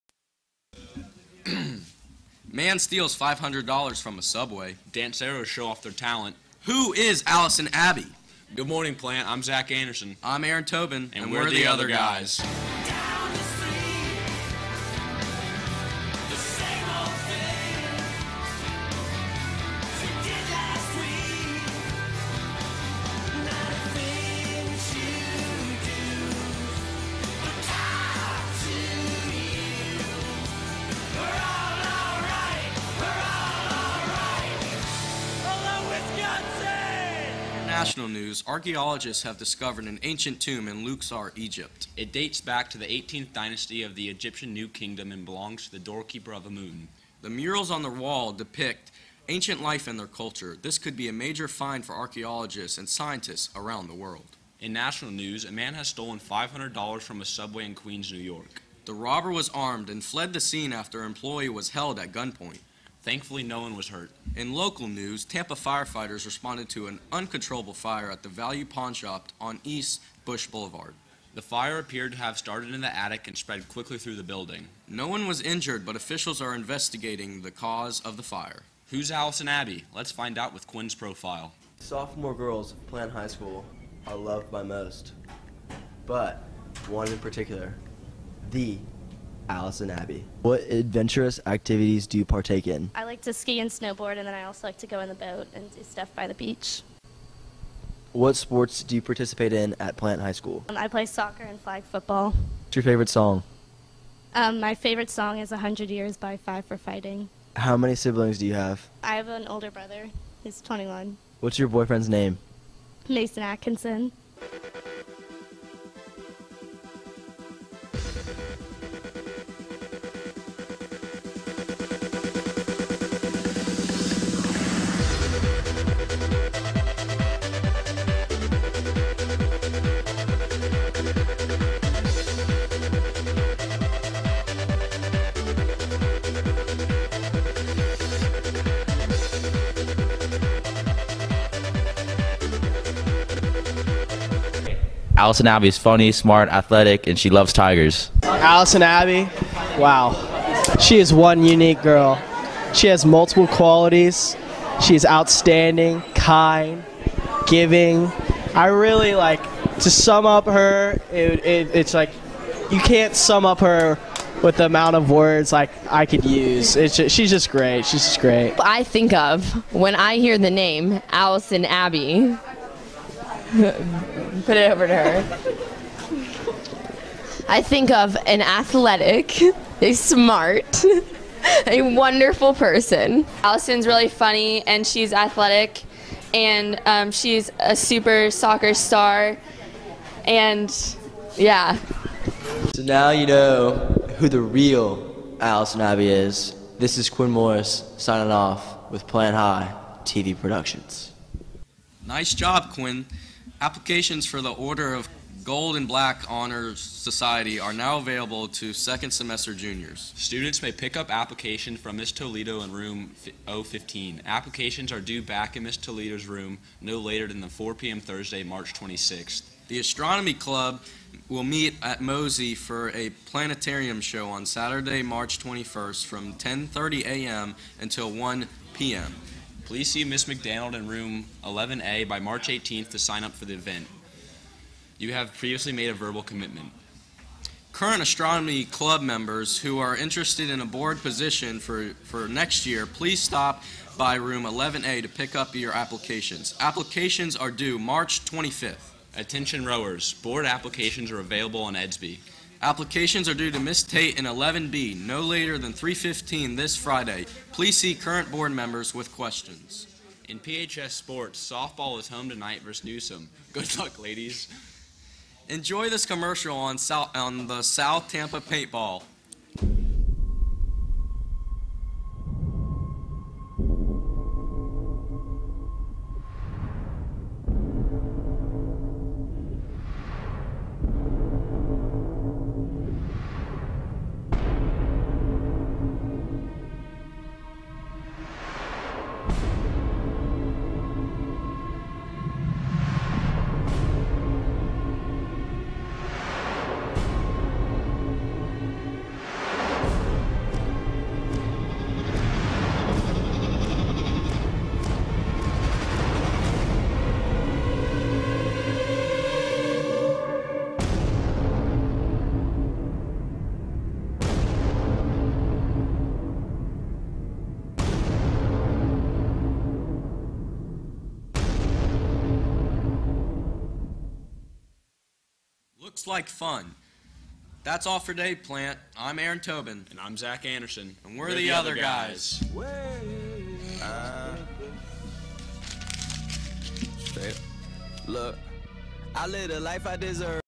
PHS Morning Show 3-19-15
Plant High Morning Show w/ Aaron Tobin & Zach Anderson for March 19, 2015.